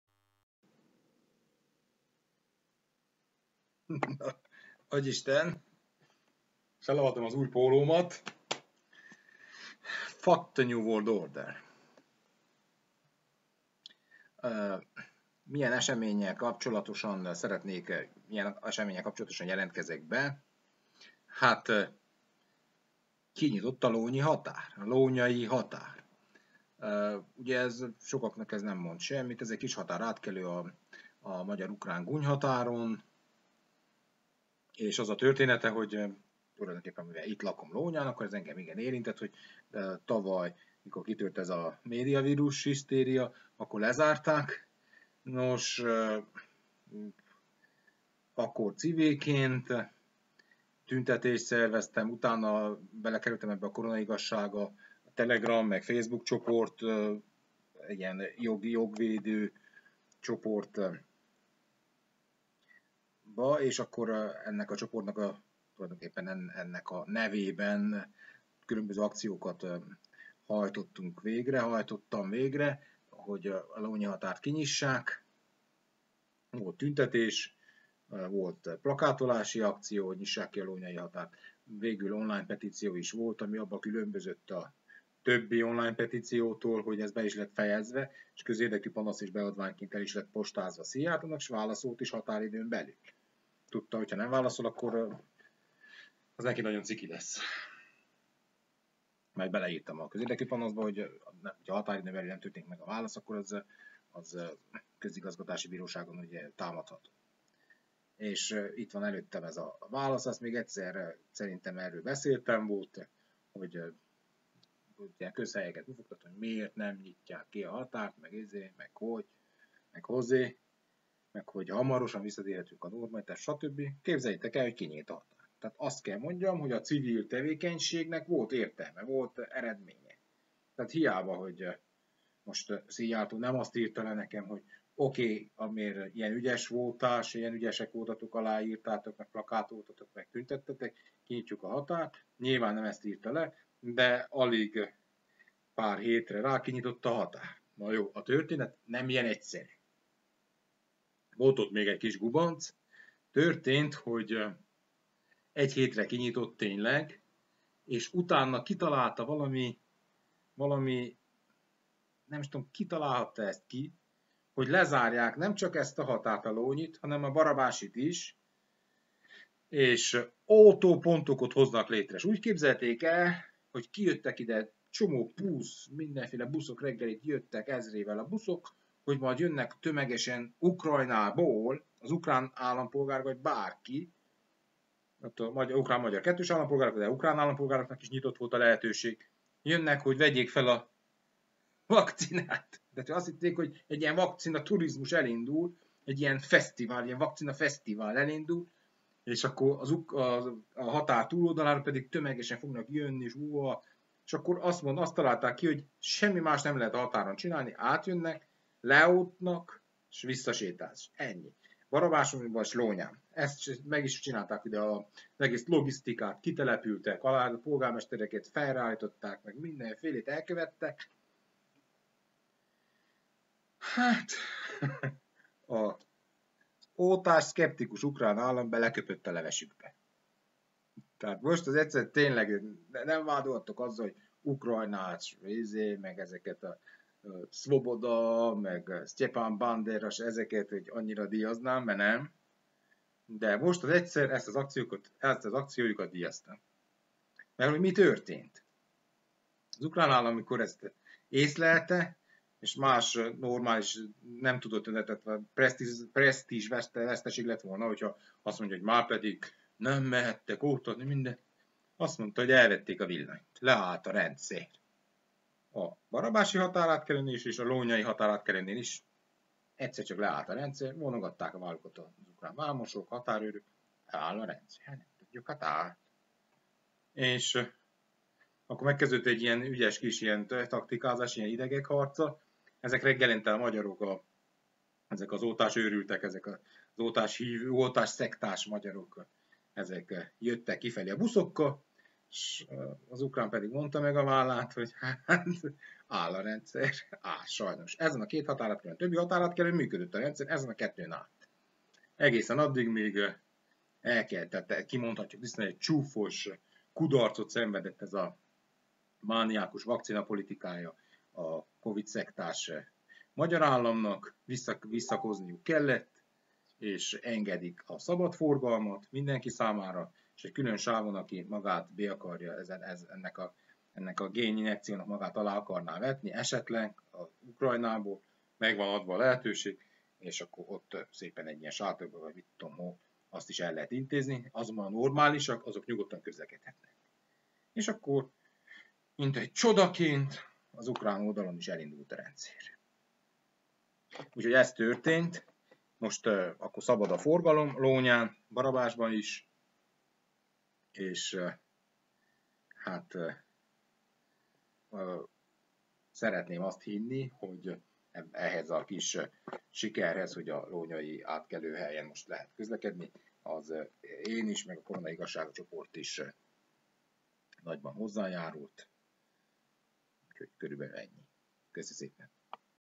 Sikeres volt a civil ellenállás - kinyitott a lónyai határátkelő
Ehhez azonban az is kellett, hogy az oltásszkeptikus ukrán állam ravasz, már-már góbés külügyi taktikai húzással alázza porig a kovidszektás, vakcinaőrült magyarokat, akik másfél éve tartották zárva a médianátha ürügyén a lónyai határátkelőt.
És ugyanitt feltehető a kérdés, főleg nacionalistáknak, hogy miért is nem oltatnak hagyományos ellenfeleink, az ukránok, tótok, szerbek, románok (mindenik trianoni utódnép körében minimális az oltakozás, mégse haltak ki, sőt, jobban vannak, mint valaha), és miért (és kik) oltják le pont a magyarokat és székelyeket ilyen elképesztően nagy arányban.